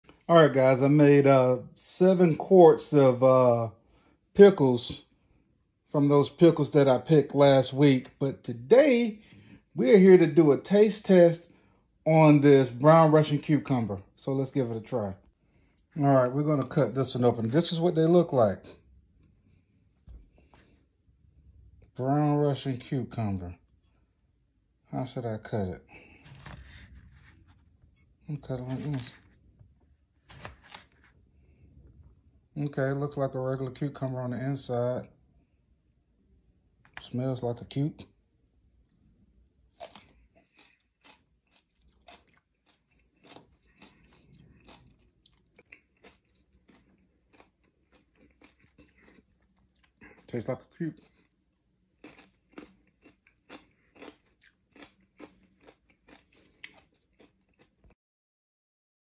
1st Brown Russian Cucumber Taste Test
Sharing my 1st taste test of a brown Russian cucumber. This is my first time growing this unique cucumber and want to share the taste comparison to other cucumbers.